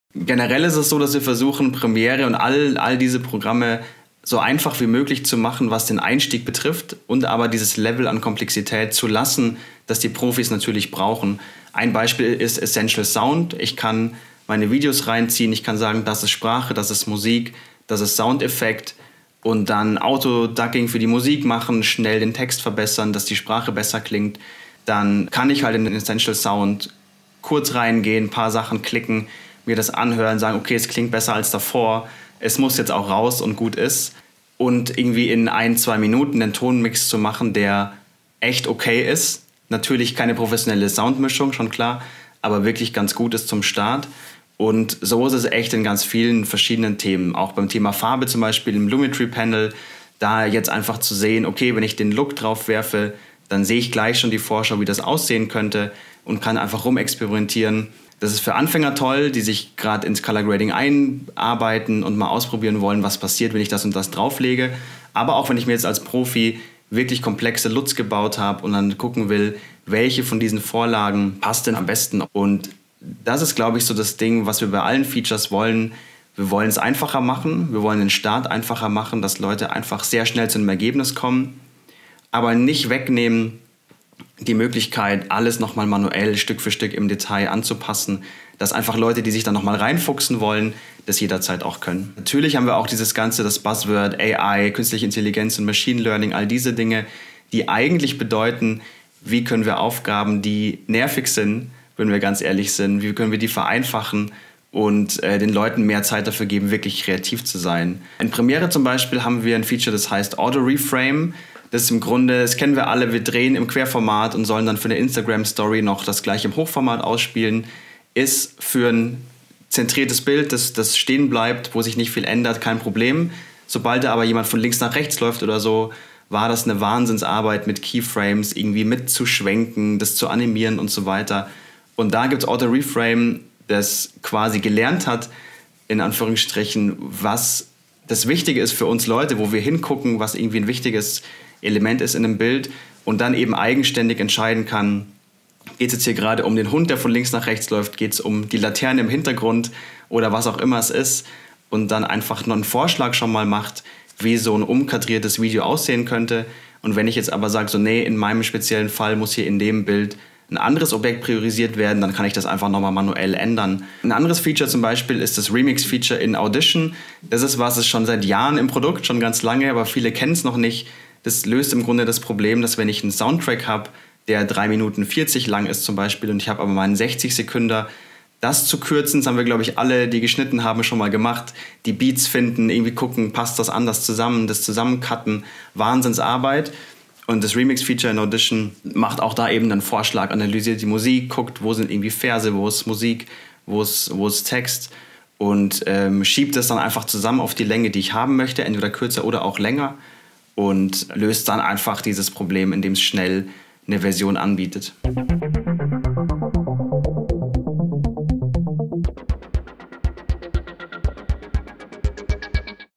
Künstliche Intelligenz im Schnitt
Künstliche Intelligenz kann im Editing helfen: etwa beim automatisierten Reframing eines Clips oder mit einfachen Möglichkeiten für Grading oder Remixes. Anton Knoblach, Product Marketing Manager Video bei Adobe, stellt im Video einige solcher Funktionen vor.